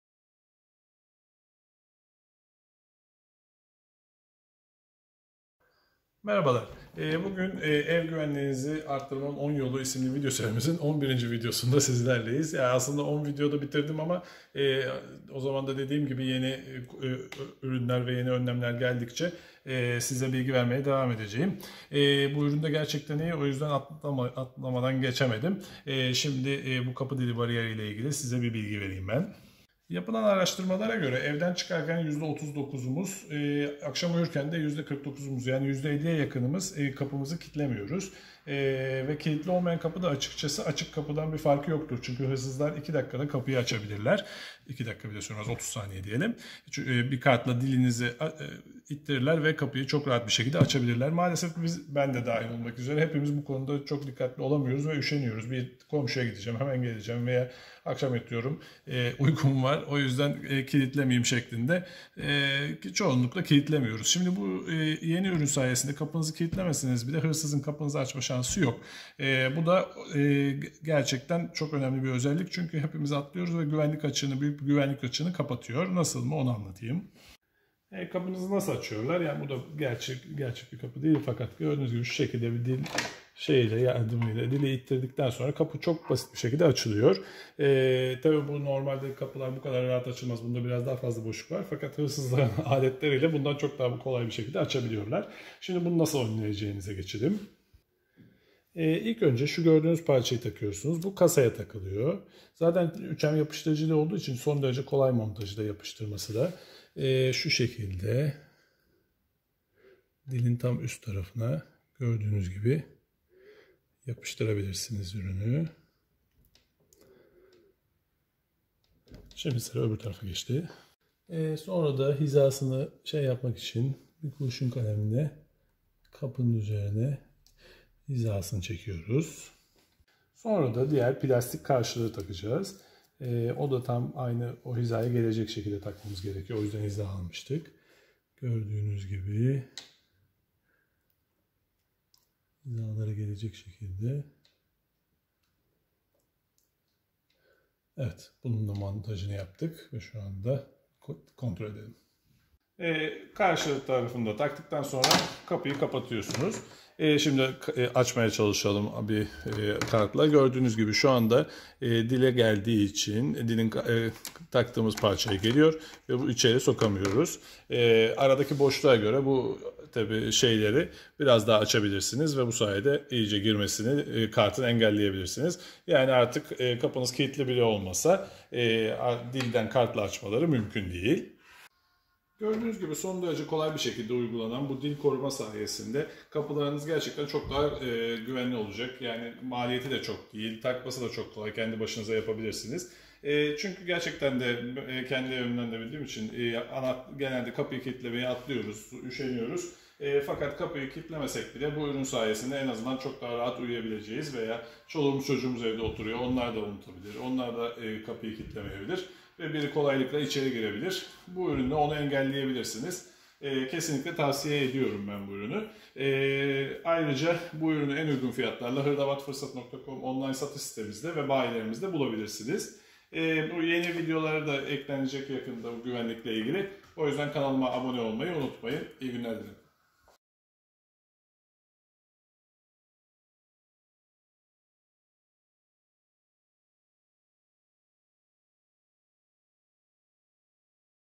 "Ev güvenliğini arttırmanın 10 yolu" - Video 11 : Çelik kapı kilit dilinize "dil koruma" yı takın!
ÇELİK KAPILAR İÇİN KARTLA AÇILMAYA KARŞI KAPI DİLİ KORUMA BARİYERİ

* Çelik kapılarda genelde kapıları kilitlemeyi unutuyoruz ve bu da büyük bir güvenlik açığına sebep oluyor. Hırsızların bir aparatla 5 sn de kapınızı açmalarına sebep olan bu güvenlik açığını önlemek artık sizin elinizde. Sektörde çığır açan bu ürün sayesinde kapınızı hırsızlara karşı çok daha kuvvetli hale getirebilirsiniz.

* Yapışkanıyla birlikte gelen bu ürünü kendi başınıza 3 dk da monte edebilir ve hemen güvenliğe geçebilirsiniz. Vidalama gerektirmediğinden tornavidaya dahi ihtiyaç duymadan monte edebilirsiniz. Bu kadar kolay uygulayabileceğiniz başka bir güvenlik önlemi yoktur.

* Şimdi Türkiye'de ilk defa yapılan kapı dili koruma bariyeri yine Türkiye'de online olarak ilk defa yine online satış sitemizde satışa sunulmuştur.

* Bu ürünü kesinlikle kullanmanızı tavsiye ederiz, çünkü yapılan güvenlik araştırmalarına göre kullanıcıların %38 i evden çıkarken, % 49 u ise gece yatarken kapılarını kilitlemediklerini söylemişlerdir. Bunu çoğu kez bizler de yapıyoruz ve evimiz hırsızlığa karşı çok açık oluyor. Kapıyı kilitlememekle kapıyı açık bırakmak arasında hiçbir fark bulunmuyor maalesef; çünkü hırsızların 10 sn ni almıyor kilitli olmayan kapıyı açmak. Bu ürün sayesinde gece çok rahat uyuyabilir ve acaba kapıyı kilitlemişler midir diye düşünmeyeceksiniz. Bu da büyük bir konfordur hem de bu fiyata alınan bir konfor.

* Şu anda kampanyada olan bu ürünü bir an önce almanızı tavsiye ederiz, çünkü kampanyamız stoklarla sınırlı durumdadır.

* Ürünün diğer iyi özelliği de kullanımının çok kolay olmasıdır, bir kere takarsınız ve sonra kilitlemekle vs uğraşmazsınız. Kapınızı çekip çıkmanız yeterlidir.

* 3M yapışkan bulunan ürünü yıllarca sorunsuz şekilde kullanabilirsiniz.

* Fiyat, 1 adet için kampanyalı fiyatıdır.

* Montajı :

1) Metal parçayı kapı kasasının üzerindeki yaylı dilin geçtiği kilit karşılığına tırnakları dışarı gelecek şekilde yapıştırınız.

2) Plastik parçayı uygulamadan önce yapıştırdığınız metal parçanın alt ve üst bitiş noktalarını kapı pervazında kalemle işaretleyiniz. Ardından plastik parçayı "ters U" şeklinde açık kısmının metalde olduğu yönün aynısına göre yapıştırınız.

3) Kapı kapandığında metal ve plastik kısımlar şekildeki gibi biraraya gelecektir.